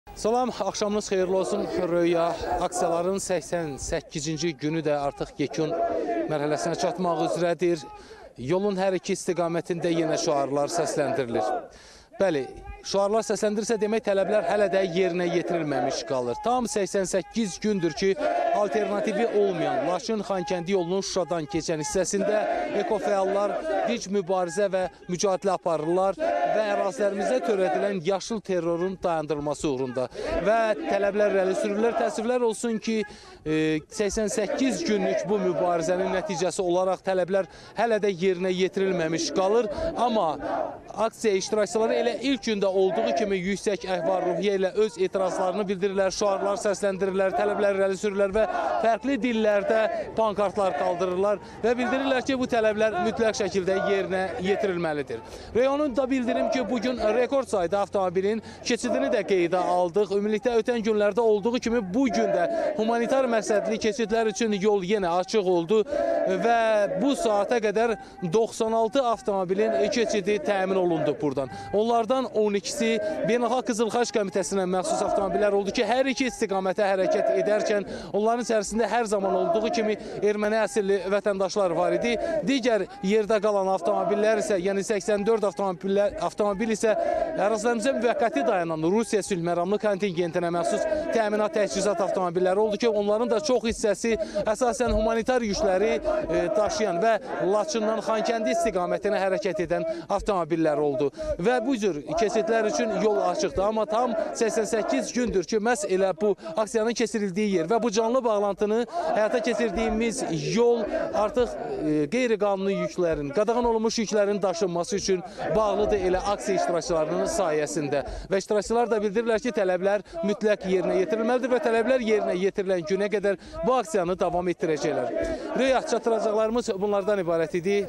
Laçın-Xankəndi yolunda dinc mübarizə davam edir - Aksiya keçirilən ərazidən BİRBAŞA BAĞLANTI
LAÇIN YOLUNDA AKSİYA DAVAM EDİR